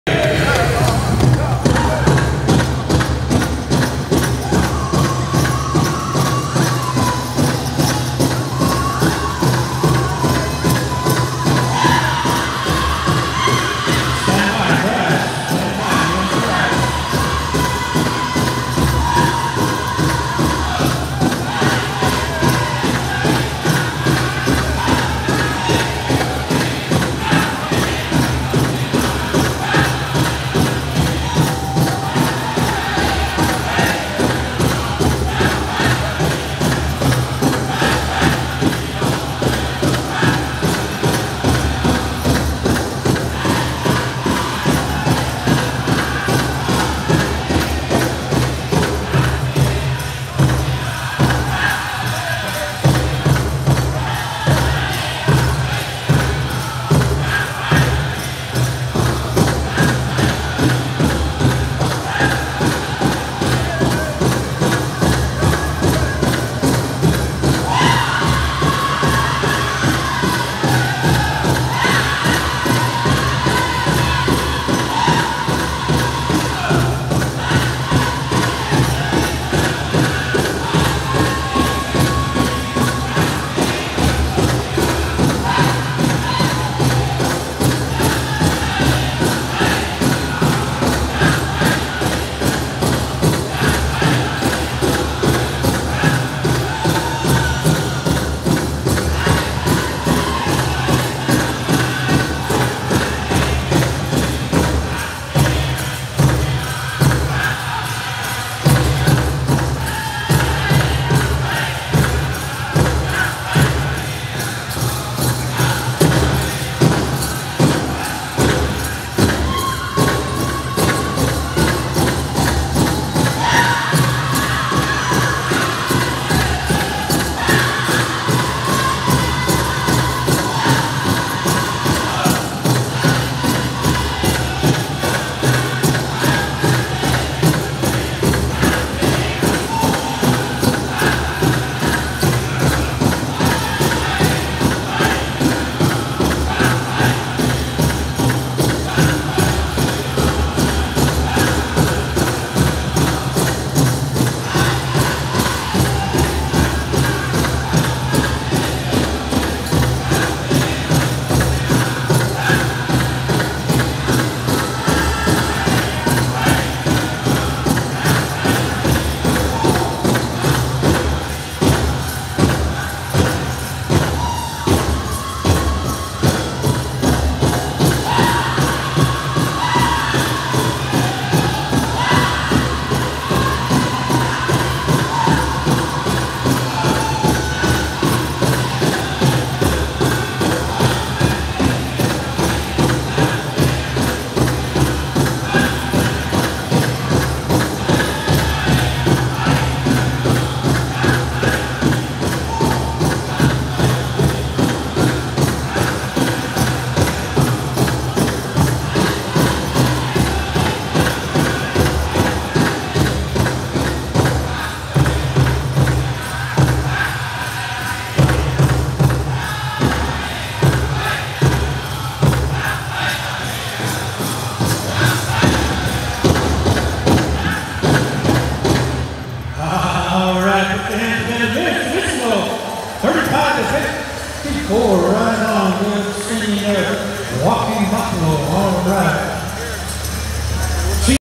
Crossroads Powwow 2019 Sr. Men's Traditional... Enjoy the Moment!
4th Annual Fargo Moorhead Crossroads Powwow 2019... Senior Men's Traditional... Fargo North Dakota, October 5th & 6th, 2019... Walking Buffalo Singers... Another video that wasn't loaded... again, a bit late, it is what it is... Enjoy the Moment... :-)